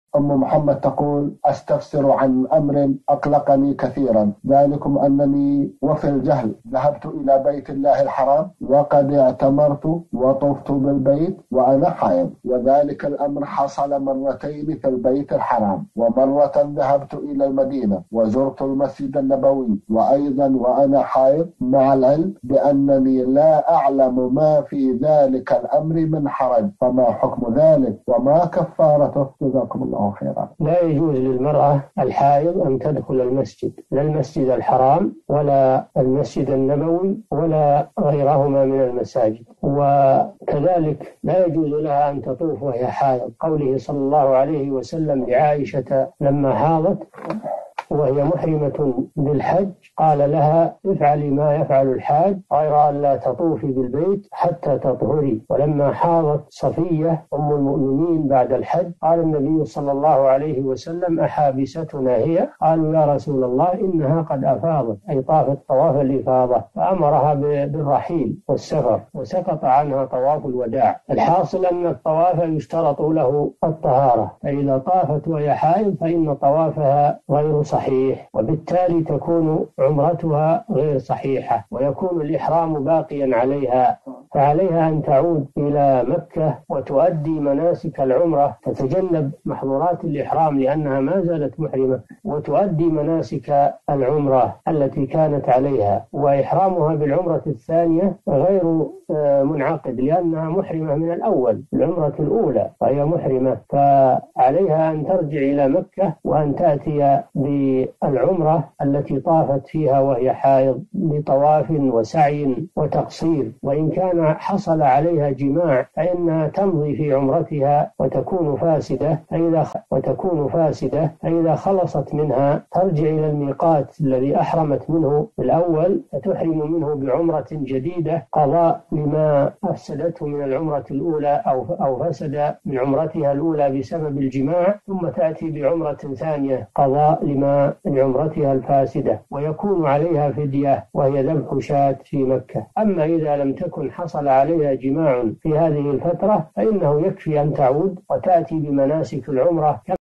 طفت واعتمرت وأنا حائض، وزرت المسجد النبوي وأنا حائض، فما حكم ذلك، ومما كفارته؟ الشيخ صالح الفوزان
نص السؤال: استفسر عن أمرٍ أقلقني كثيرًا ذلكم أنني وفي الجهل ذهبتُ إلى بيت الله الحرام، وقد اعتمرتُ، وطفتُ بالبيت، وأنا حائض، وذلك الأمر حصل مرتين في البيت الحرام، ومرةً ذهبتُ إلى المدينة، وزرتُ المسجد النبوي وأنا حائض مع العلم بأنني لا أعلم ما في ذلك الأمر من حرج، فما حكم ذلك، وما كفارته؟

السيرة الذاتية :
للاشتراك في قناة الشيخ صالح الفوزان

رئيس الفريق
لمشروع كبار العلماء